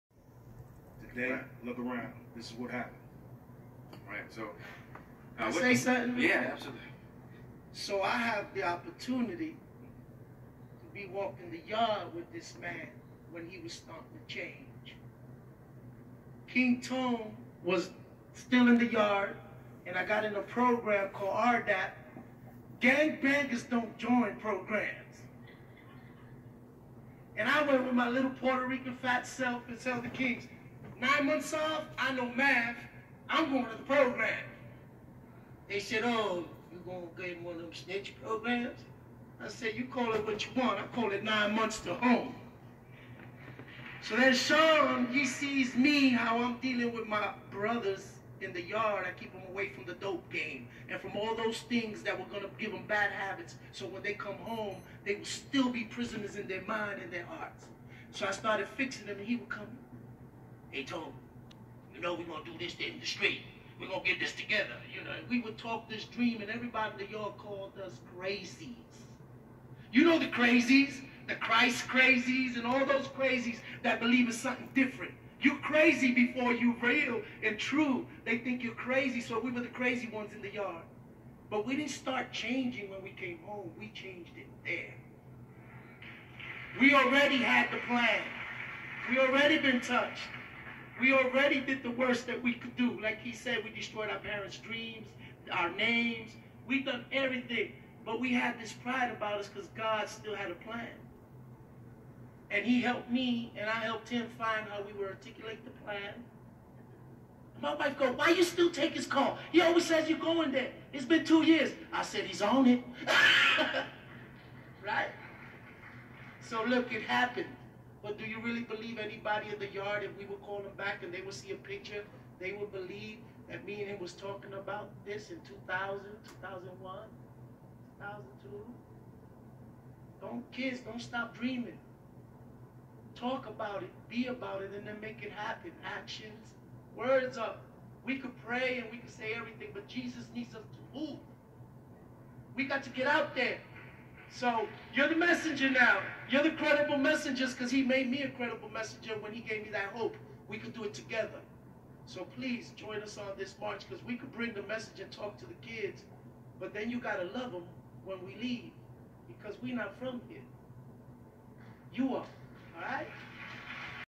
Sean Stevenson made his dream a Reality
Sean Stevenson started the end the violence while on the inside.